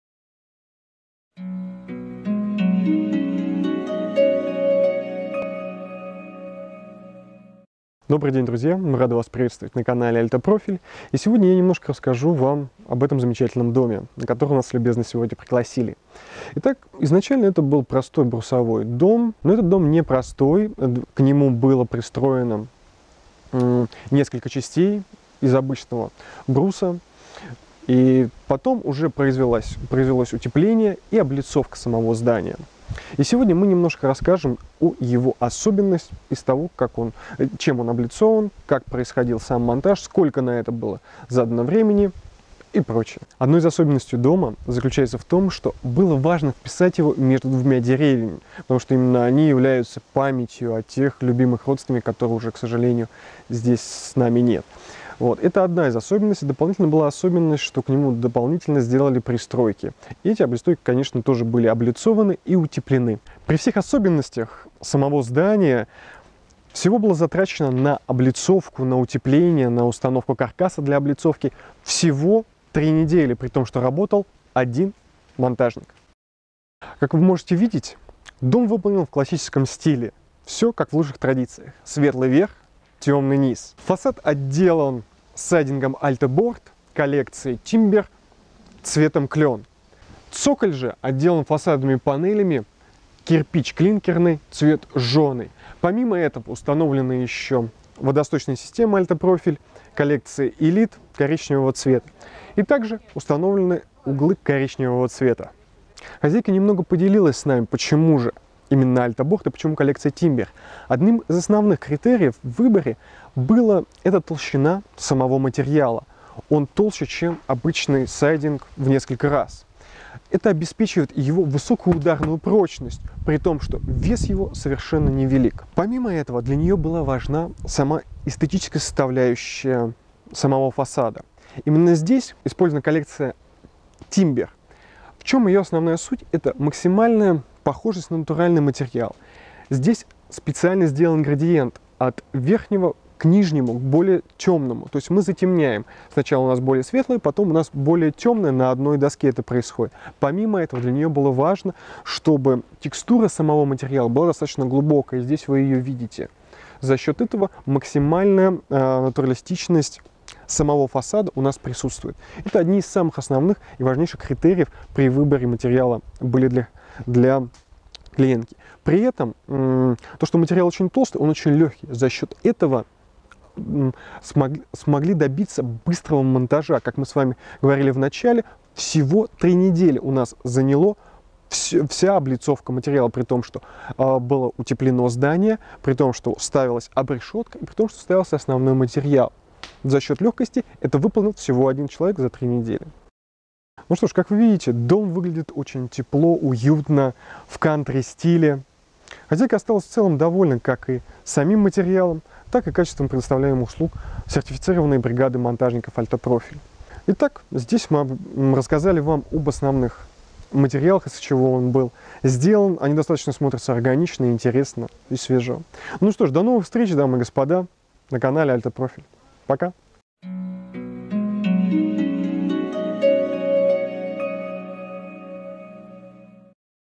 Вспененный сайдинг под доску Альта Борд (AltaBord).
Новое поколение сайдинга AltaBord, которое сочетает ударопрочность и рельефность фасадной панели и гибкость классического сайдинга.

Для отделки фасада дома из данного видеоролика использованы следующие материалы:

▶ сайдинг коллекции "АЛЬТА БОРД" Тимбер Клен;
▶ цоколь фасадные панели Кирпич Клинкерный жженый;
▶ водосточная система Элит коричневая.

Из видеоролика: выбор коллекции заказчиком обусловлен натуралистичностью, которая достигнута за счет цветовых градиентов, глубиной рисунка, толщиной и легкостью панели.

Монтаж выполнен одним мастером за 21 день.